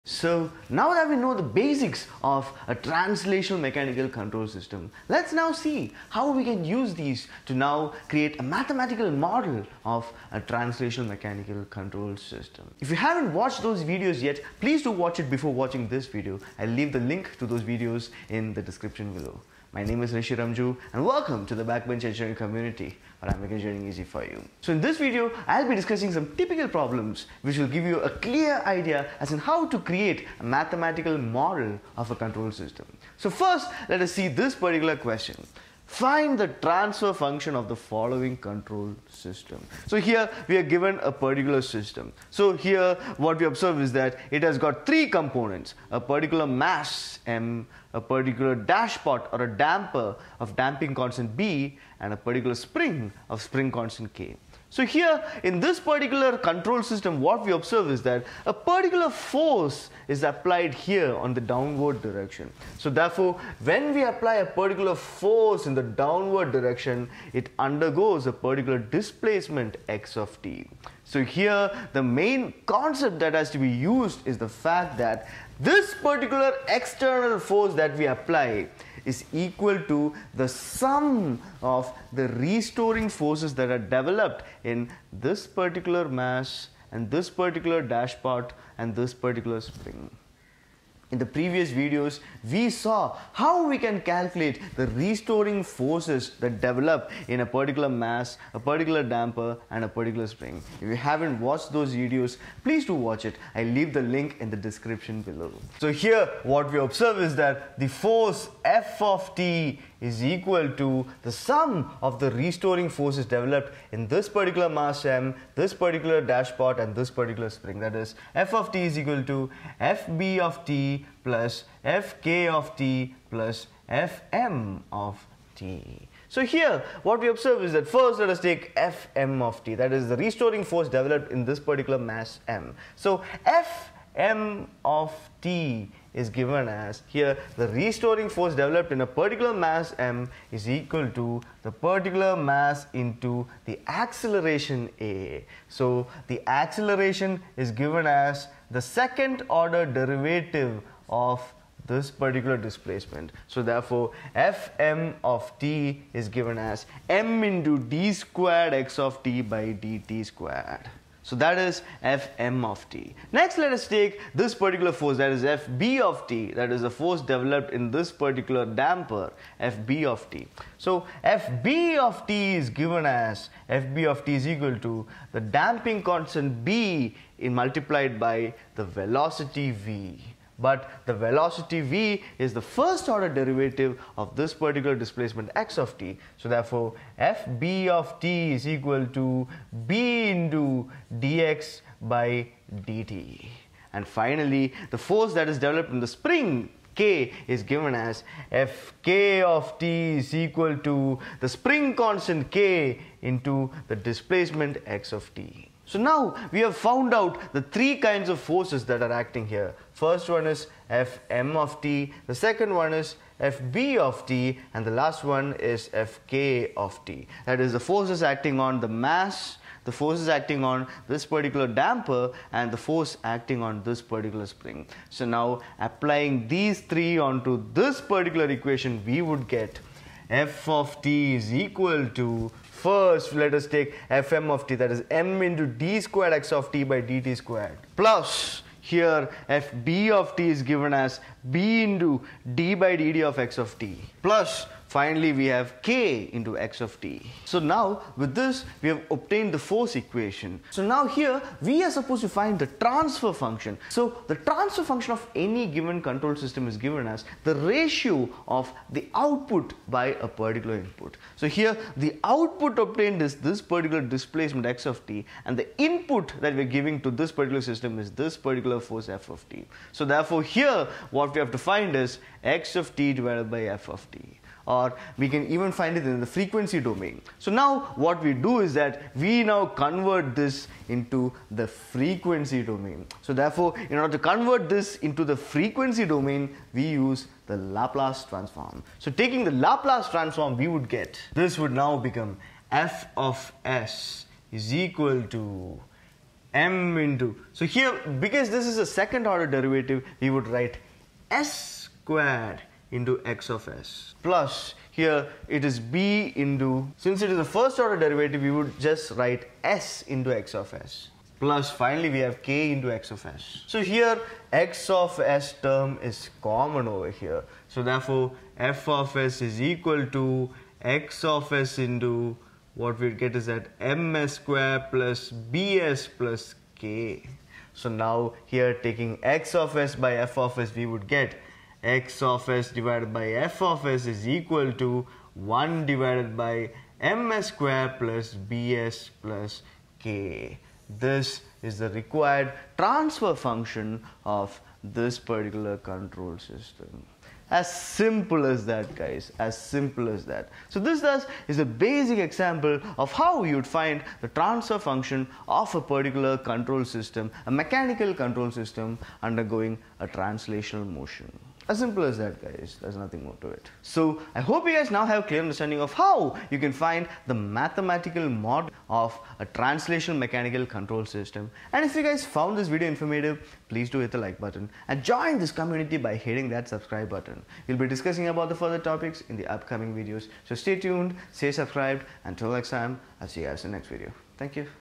Mathematical Modelling of Translational Control Systems | Finding Transfer Functions | Simplified
1. Writing the force equation of a Translational Mechanical Control System.
2. How to find the transfer function of a given translational mechanical control system using mathematical modelling of the control system.

I hope you guys found this video informative and now have a clear idea of what you mean by Mathematical Modelling of Translational Mechanical control systems. We'll be discussing about the further topics in the upcoming videos. Together let’s grow.